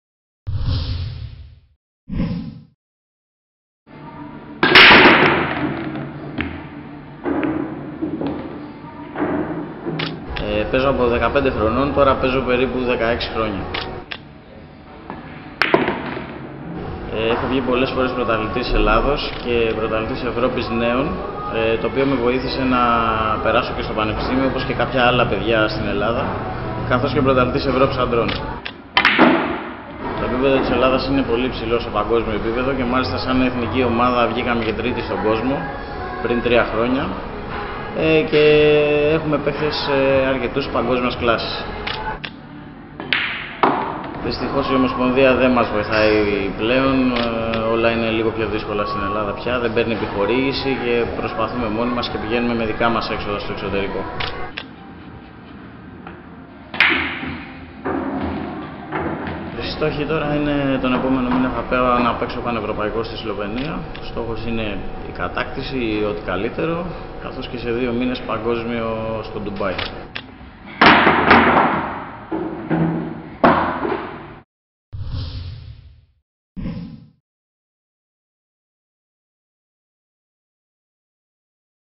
Η καλή στεκιά πληρώνεται ακριβά
Ο πρωταθλητής Ευρώπης στο μπιλιάρδο, Νίκος Οικονομόπουλος, μιλάει για το αγαπημένο του άθλημα

Δημοσιογράφος: Αλεξάνδρα Τζαβέλλα
Εικόνα: Ορέστης Σεφέρογλου
Μοντάζ: Αμαλία Πορλίγκη